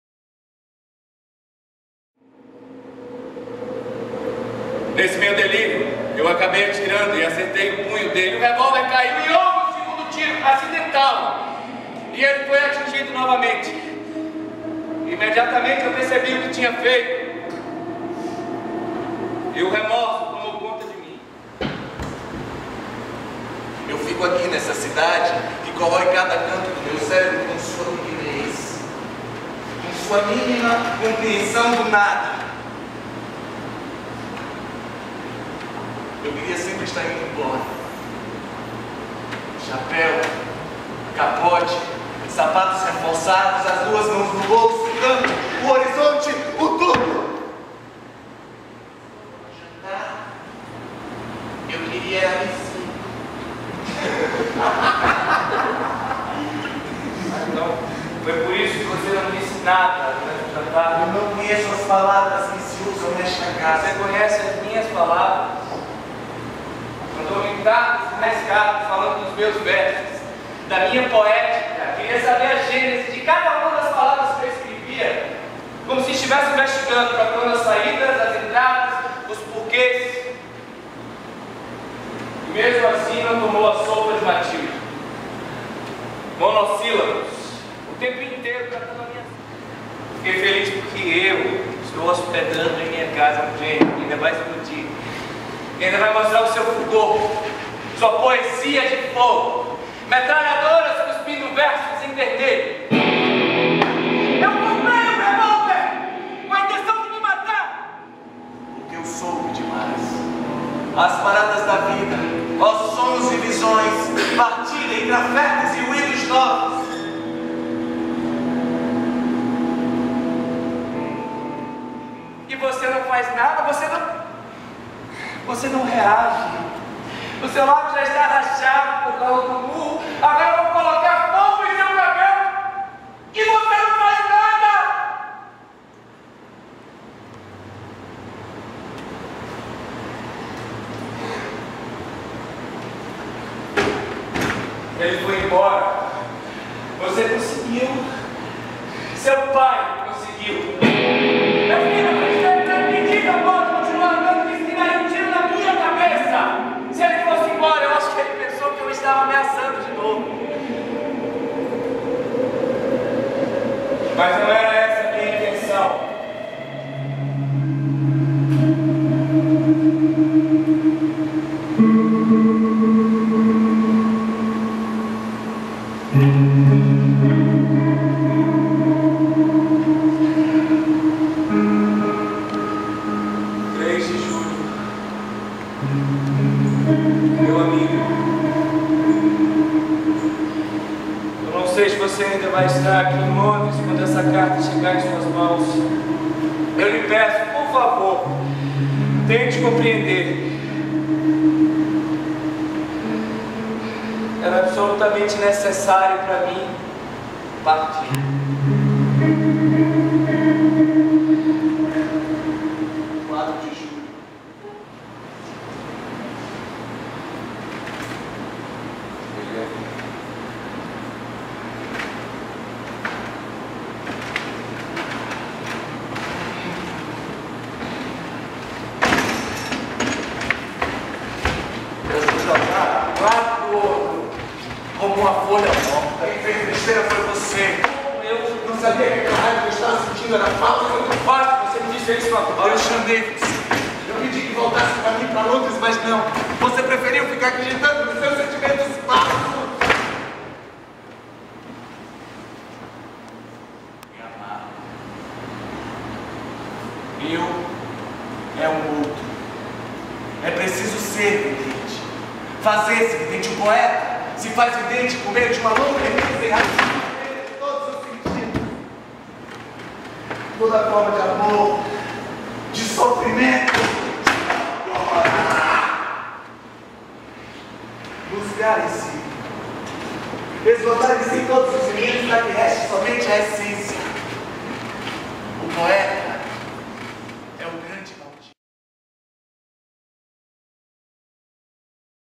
Pólvora e Poesia - Hiperativa Comunicação e Cultura (BA) - Festival Palco Giratório - Cuiabá-MT
PÓLVORA E POESIA
Hiperativa Comunicação e Cultura (BA)

Pólvora e poesia é um espetáculo que vai alem da historia amorosa e estética de encontros e desencontros entre Arthur Rimbaud e Paul Verlaine, dois grandes escritores do final do século XIX. Rimbaud, um poeta rebelde, de ideologias que fervilham originalidade e personalidade, esfacela a todo instante o discurso socialmente arranjado de Verlaine. Com um texto impactante, a carga poética da peca fica por conta da violência emocional com que o lirismo salta dos discursos travados para tomar forma em movimentos corporais pontuados pelos riffs de guitarra. O espetáculo convida a plateia a uma imersão no que há de mais humano: o encontro conflituoso com o próprio eu.

Apresentado em:
26/05 - 20h - Salão Social - SESC Arsenal
Gênero: Drama
Classificação etária: 18 anos
Duração: 60 minutos

(Imagens: Jan Moura)